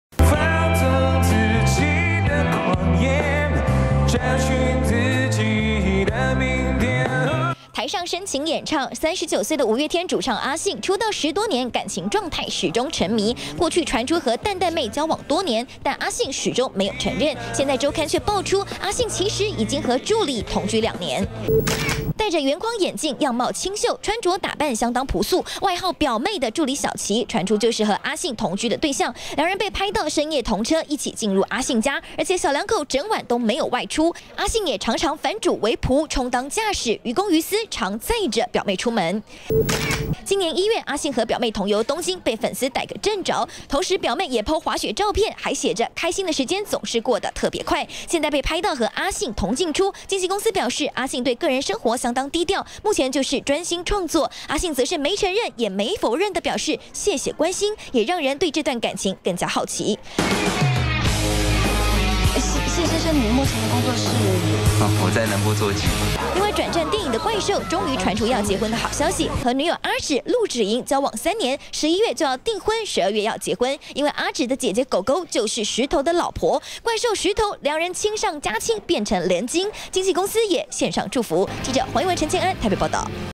20151118中天新聞 助理變愛人 週刊爆阿信同居「表妹」2年
五月天團員一個接著一個結婚，39歲的主唱阿信卻始終沒有公開承認對象，出道的十多年間，被傳出是同志，也曾經被拍到和蛋蛋妹出遊舉止親密，但卻始終沒下文。現在阿信被拍到跟助理，綽號「表妹」的小奇，深夜同車進出阿信的家，爆出兩人其實已經同居2年的消息。